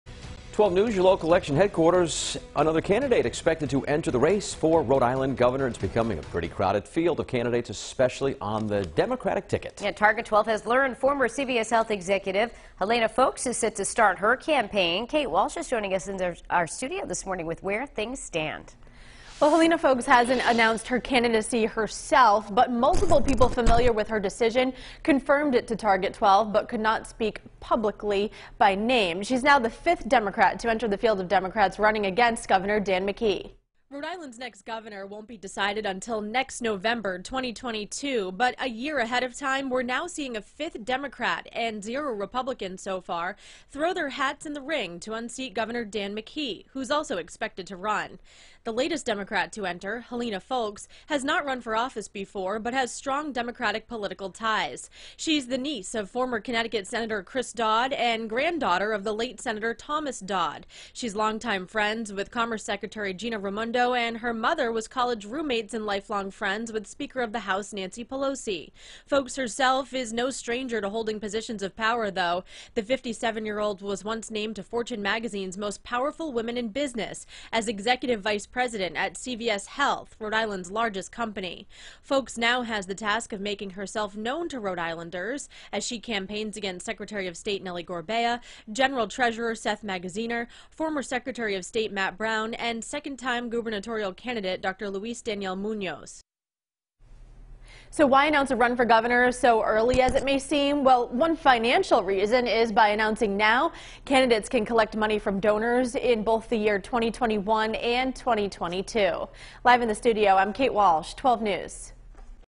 Helena Foulkes plans run for RI governor
Former CVS Health executive Helena Foulkes is planning to enter next year’s Democratic primary for Rhode Island governor, 12 News has learned, adding a deep-pocketed business leader to an already crowded field of contenders seeking the job.